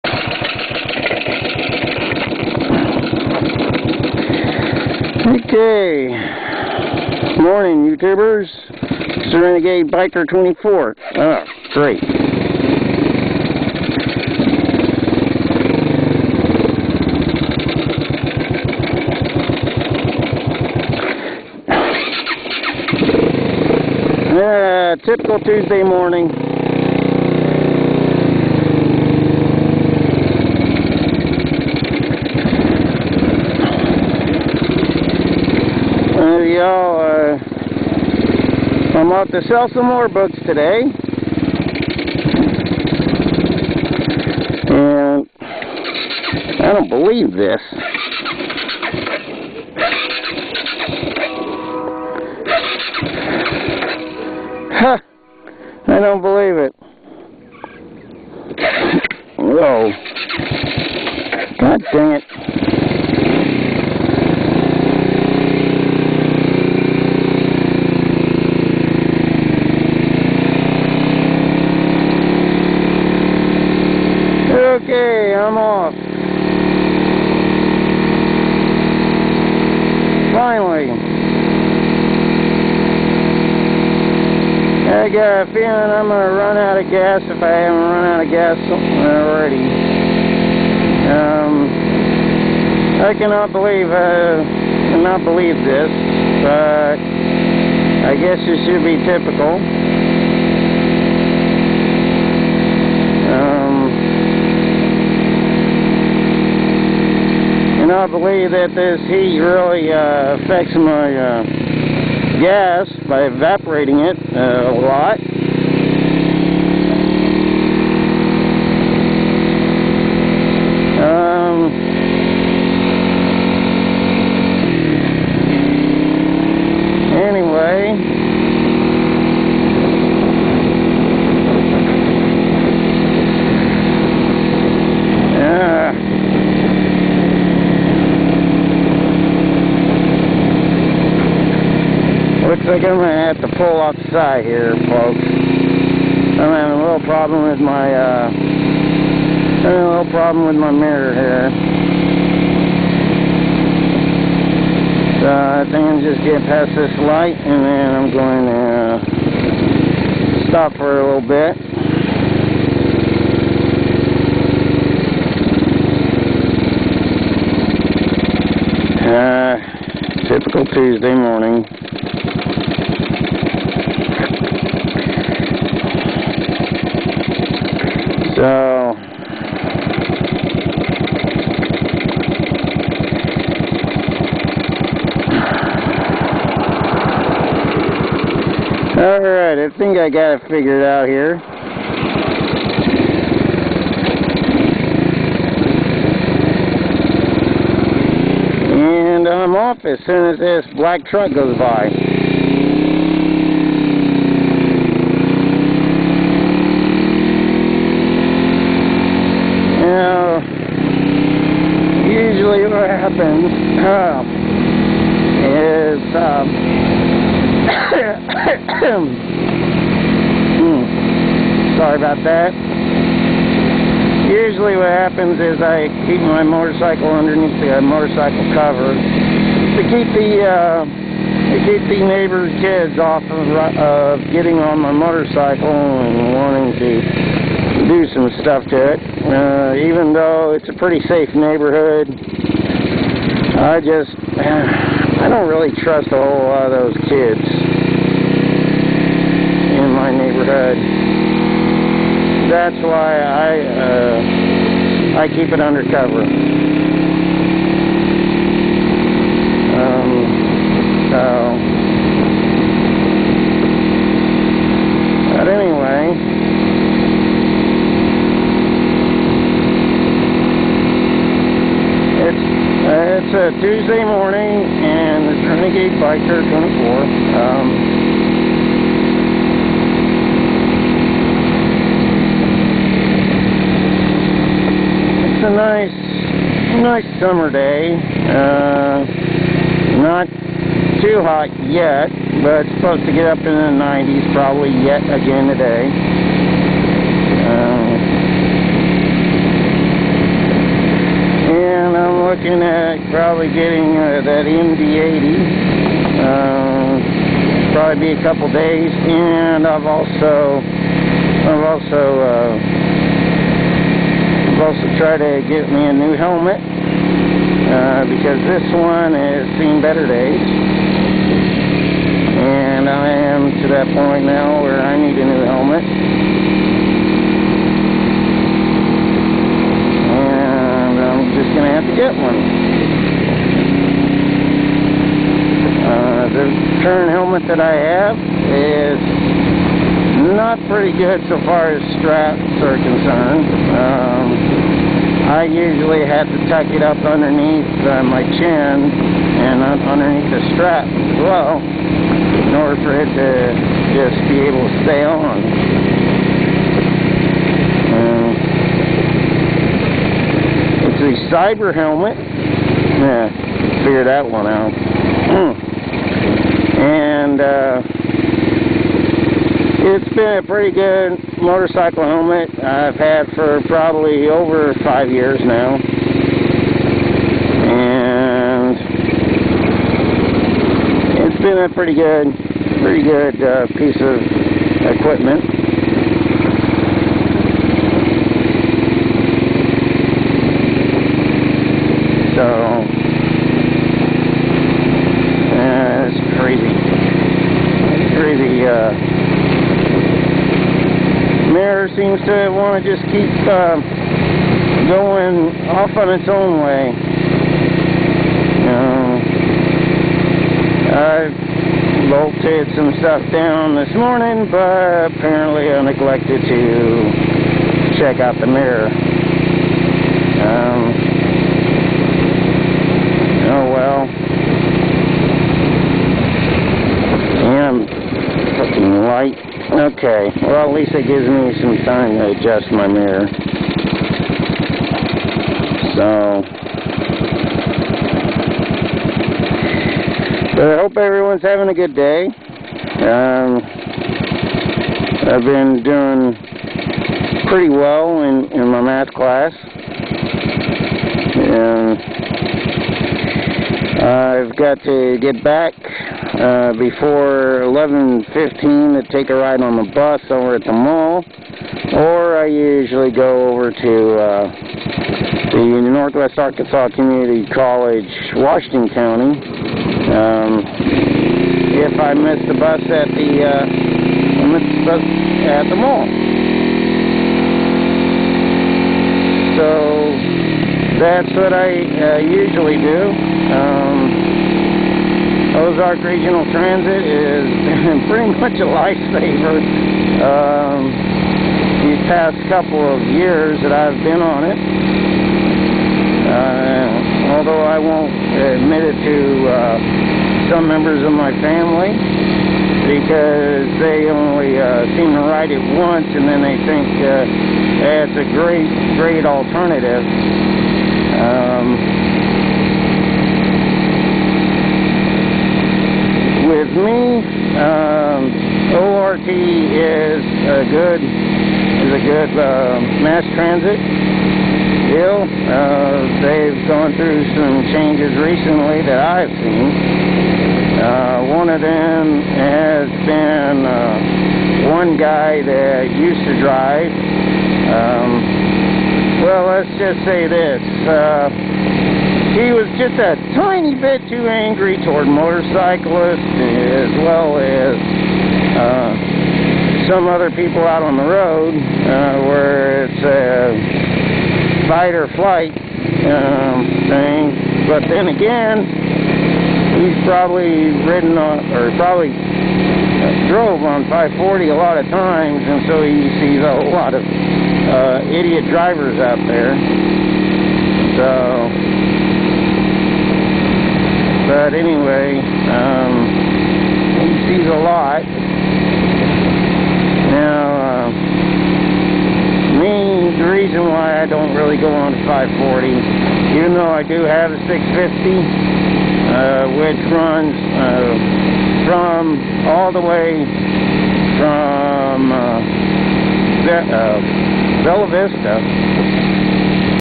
Motorcycle Problems Revisited 6-7-2011
This is about my motorcycle problems that started today. Please excuse some of the language. I was very upset at the time. Not only did it start this morning, it continued all throughout the day. I am lucky I made it home, even after all this.
I will probably be able to purchase another motorcycle after I am done with college.